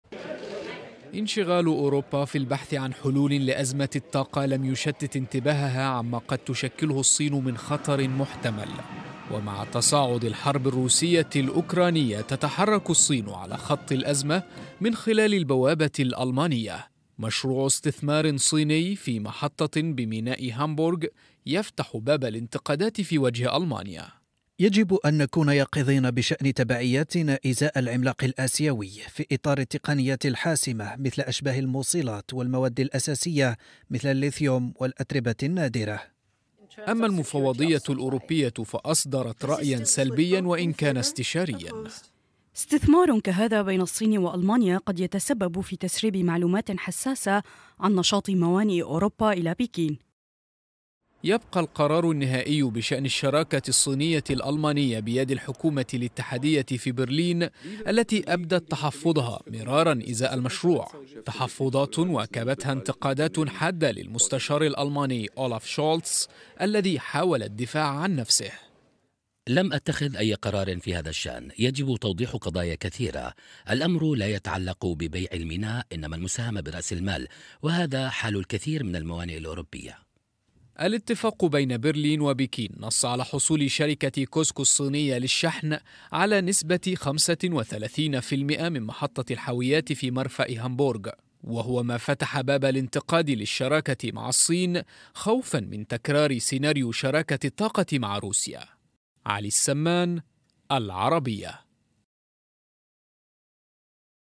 الاستثمارات الصينية تفجر خلافًا بين ألمانيا والمفوضية الأوروبية.. لماذا؟
الاستثمارات الصينية في ميناء هامبورغ تثير جدلا داخل ألمانيا.. والمفوضية الأوروبية: قد تؤدي لتسريب معلومات حساسة إلى